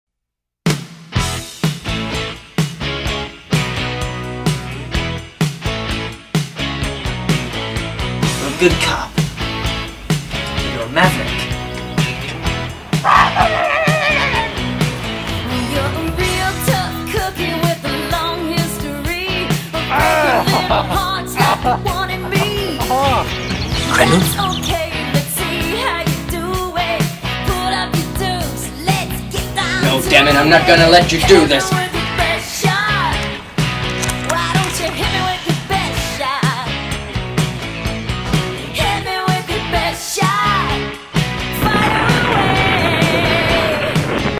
Krendal Phillips 1:X The Same Generation - Trailer
Trailer for the fourthcoming new series of Krendal Phillips Adventures. This new premier includes more action, more guns, more laughs, and more general bad assness. Rated M for mature and banned in most states but thats hardly surprising as they aren't even allowed to drink till 21 or some shit.